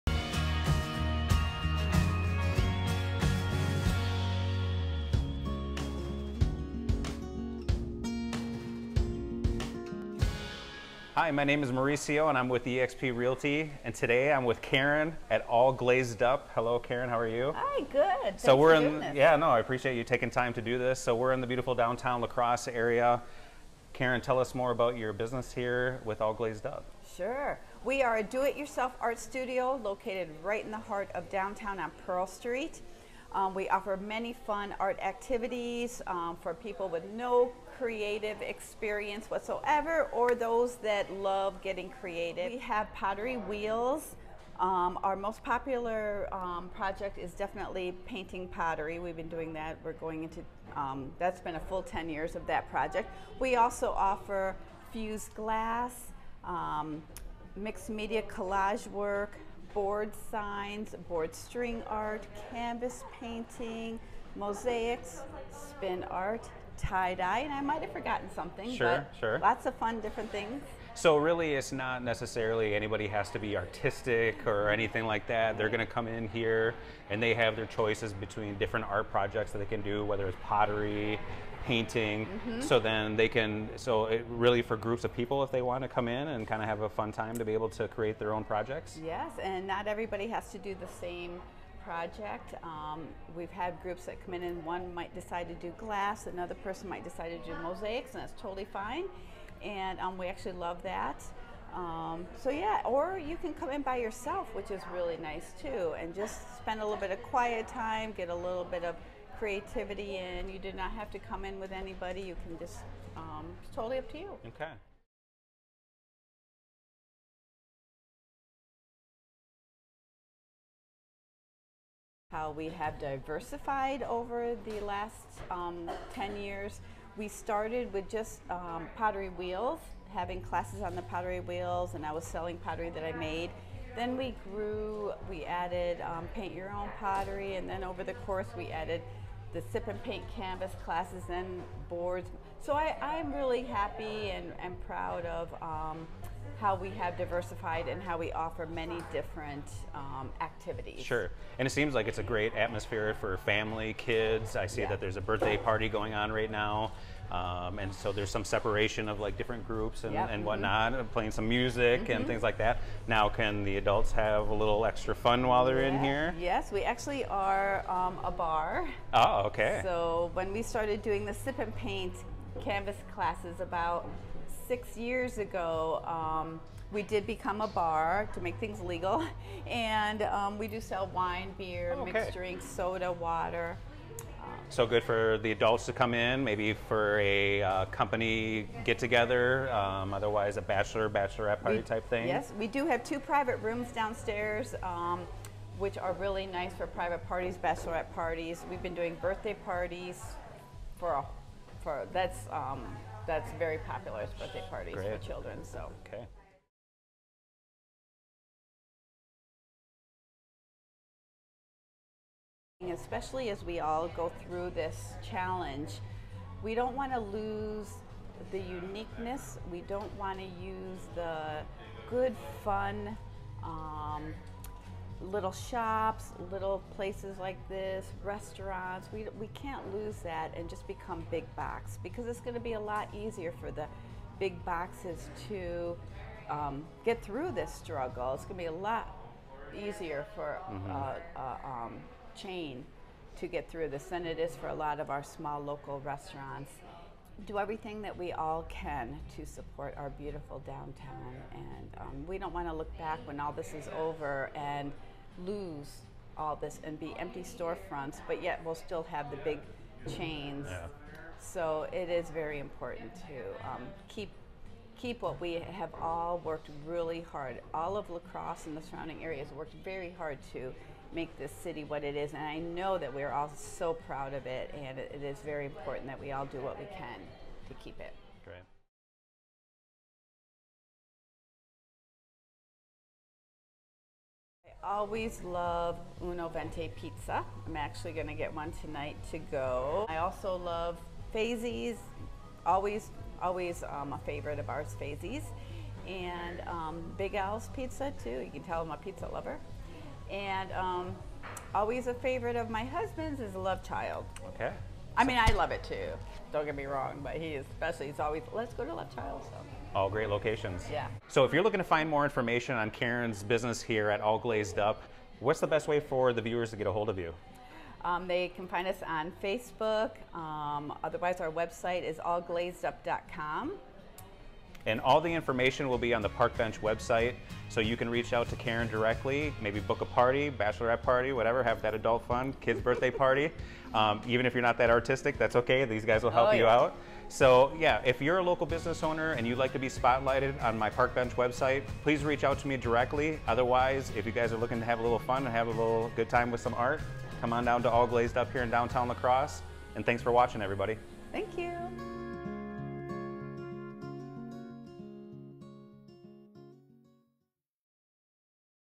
Meet Karen Bressi, Owner of All Glazed Up on Pearl St, La Crosse
1. Tell me more about your business -

We are a do it yourself art studio located downtown La Crosse on Pearl Street. We offer many fun art activities for people with no creative experience. Or for those who are creative, we offer something for you, too. We have pottery wheels, we offer fused glassed, tie-dying, mixed media collage work, board art, canvas painting, mosaics, painting pottery is very popular. There are many fun things for everyone!

One thing to note, not everyone has to do the same project. We've had groups come in where one person might want to do glass, another wants to do mosaics and that's totally fine. If someone wants to come in by themselves, that's great as well.

2. What has changed in your business -

We have really diversified over the past 10 years. We first started with offering just classes on the pottery wheels and selling the pottery I made. We then grew with adding painting your own pottery, later on down the road we added sip and paint with canvases, and then shortly after we added boards. One thing people may not know is that we also have a bar. When we started the sip and paint classes, we did become a bar. We sell wine, beer, mixed drinks, soda, water. We can host private parties, we have two large rooms downstairs designed for that.

3. Why shop local -

Going thru this challenge, especially with what this past year has brought, we don't want to lose the unique-ness, fun little shops, or restaurants. We can't lose those to the big box places. We need to do everything we can to support small businesses. We don't want to look back after all of this and see many empty store fronts because we have all worked so hard to get to where we are.

4. What are your favorite restaurants -

I always love Uno Venti Pizza, I'm actually going to get one tonight. Fayzee's is a local favorite, Big Al's Pizza, and Love Child is a favorite of my husband.

5. What's the best way for viewers to get a hold of you -

Note: This article is about All Glazed Up. Please visit their profile page to find more information and review the business.